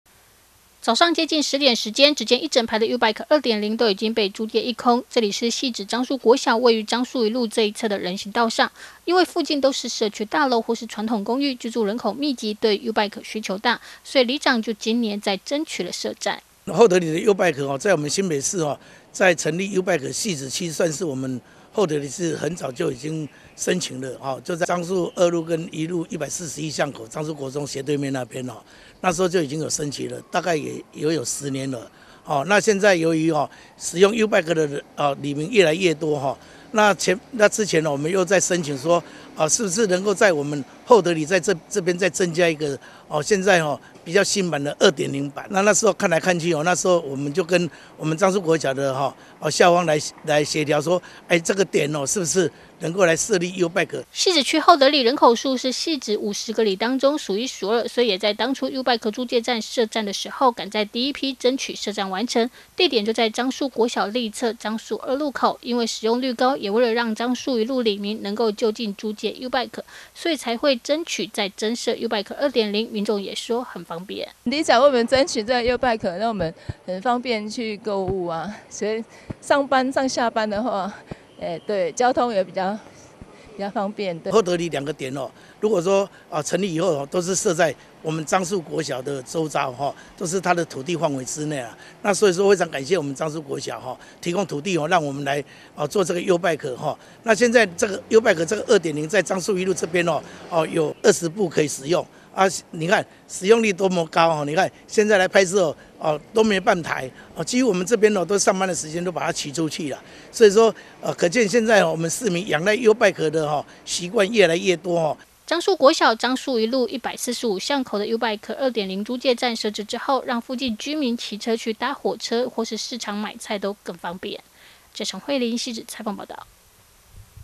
20230609觀天下新聞 Youbike使用率高 厚德里再設2 0租借站
汐止區厚德里人口數可以說是汐止50個里當中數一數二，居住人口多一直以來對於youbike的需求也大，所以，里長陳有諒也再次爭取，在樟樹國小的樟樹二路側，增設youbike2.0租借站，完成後使用率超高，經常早上都租借一空。